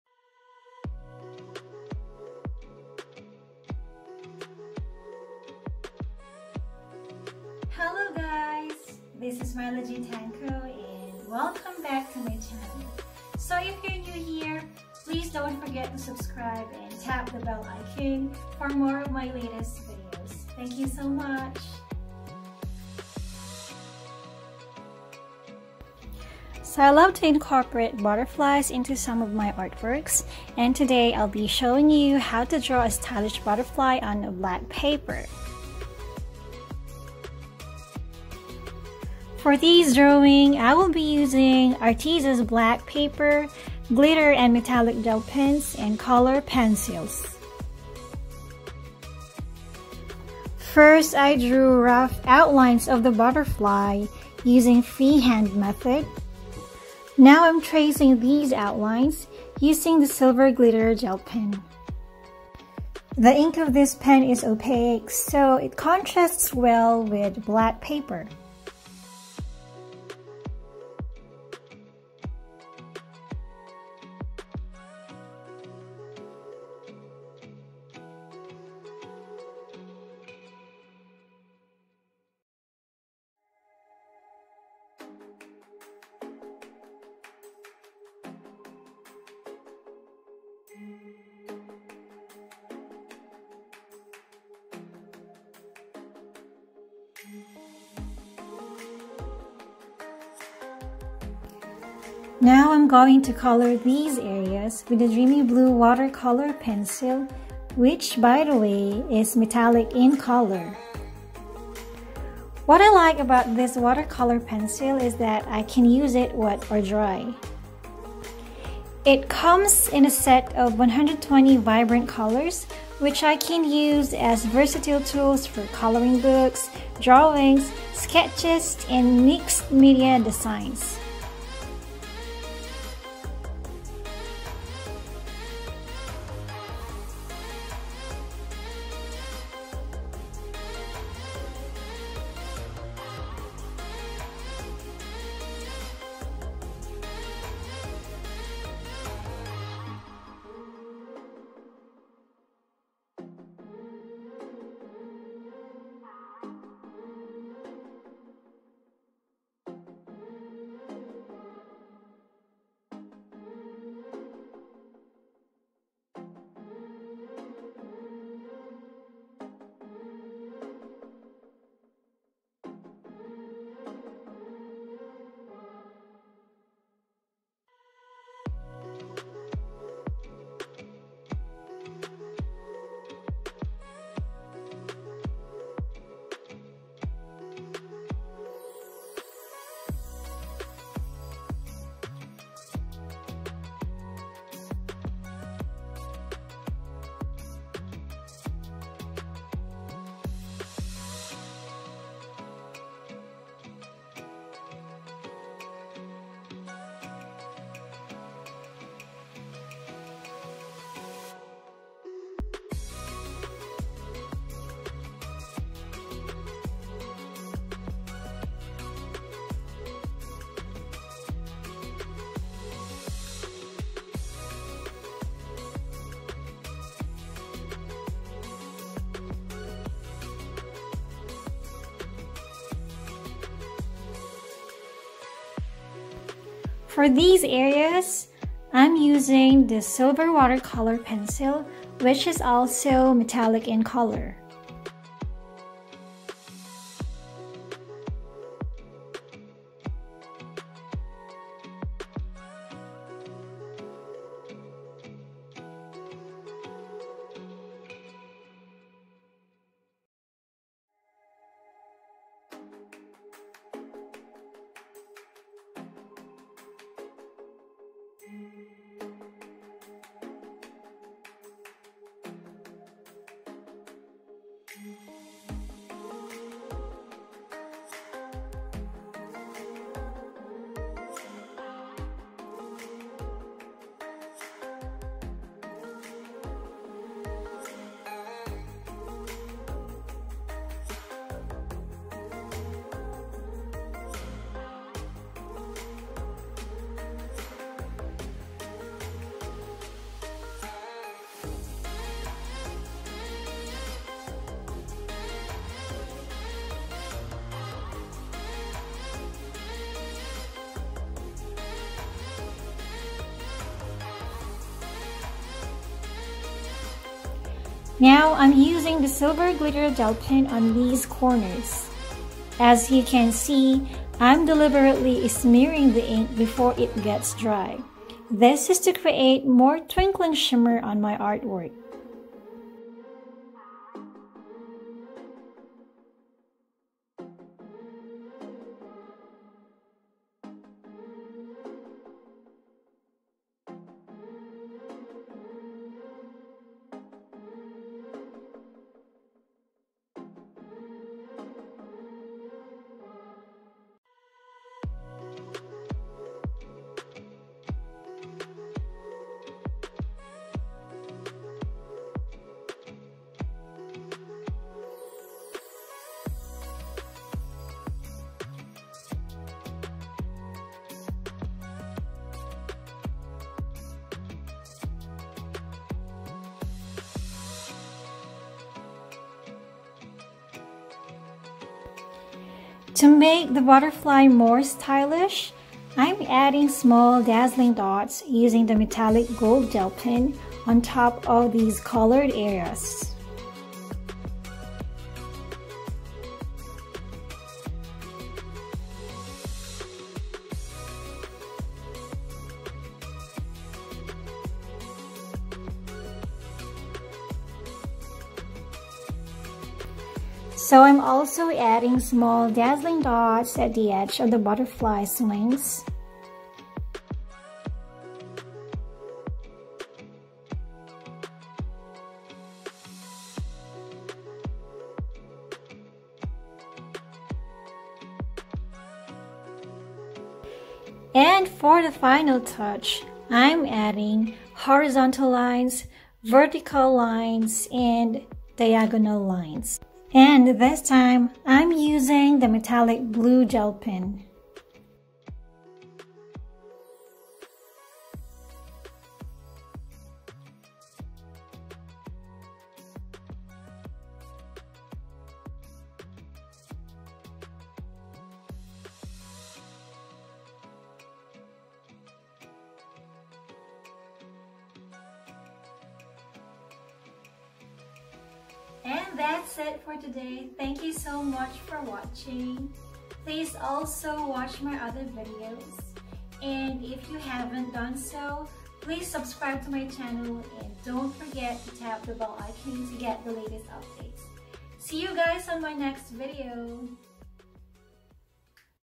HOW TO DRAW STYLISH BUTTERFLY ON BLACK PAPER/ STEP BY STEP
Love shimmers on your artwork? Here's how to draw a stylish butterfly on a black paper using glitter and metallic gel pens and watercolor pencils!😍

Materials:
• Arteza black paper
• Arteza gel pens (silver glitter, metallic gold, metallic blue)
• Arteza professional watercolor pencils (silver, dreamy blue, sapphire blue)